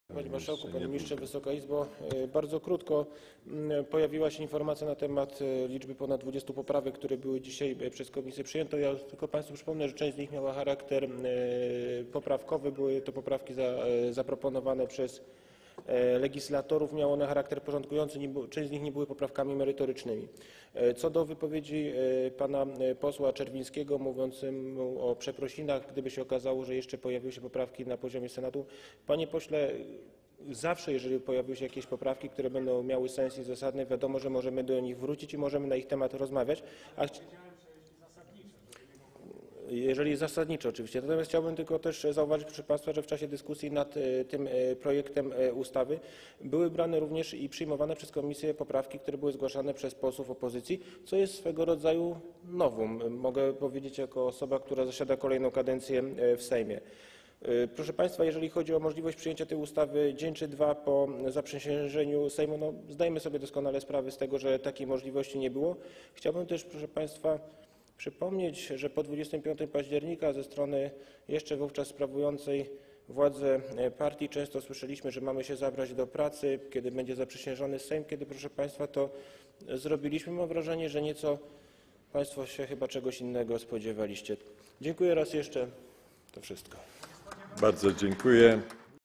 Wojciech Zubowski - wystąpienie z 9 lutego 2016 r.
Popierasz? Udostępnij! Wojciech Zubowski - wystąpienie z 9 lutego 2016 r.
11. posiedzenie Sejmu RP

Sprawozdanie Komisji o poselskim projekcie ustawy o zmianie ustawy o działach administracji rządowej oraz niektórych innych ustaw